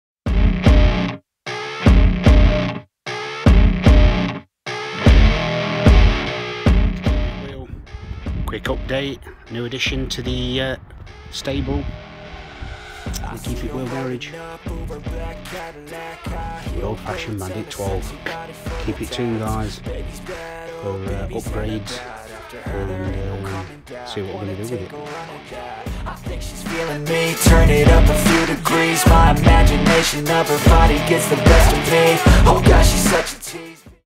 Project Bandit Intro
New addition to the keep it wheel stable. suzuki gsf 1200 bandit.
Keep upto date with this new series as we turn this bike into an old school streetfighter!Project Bandit!